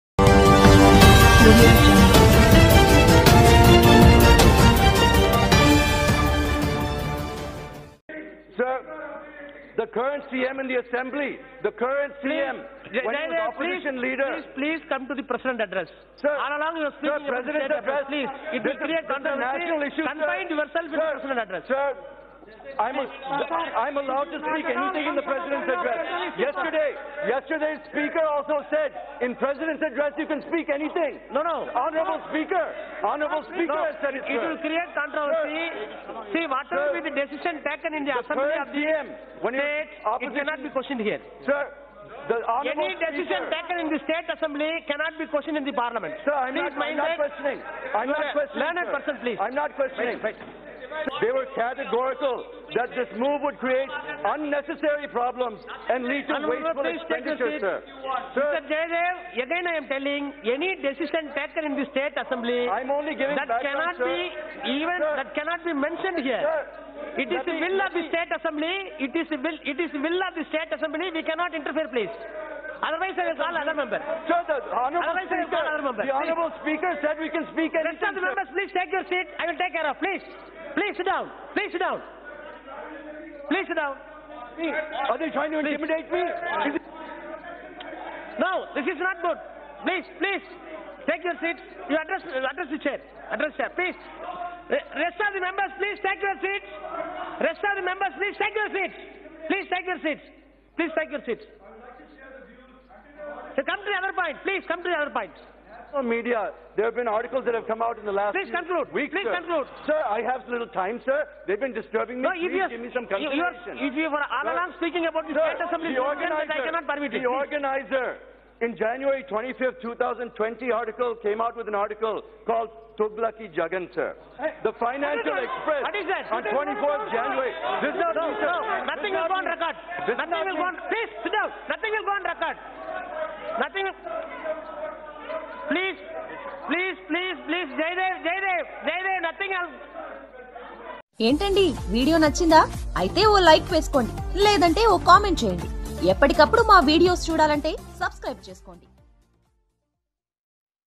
Tdp Mp Galla Jayadev Fires On Ys Jagan And Spikar At Lok Sabha | Lok Sabha Hilights | Fata Fut News
Watch#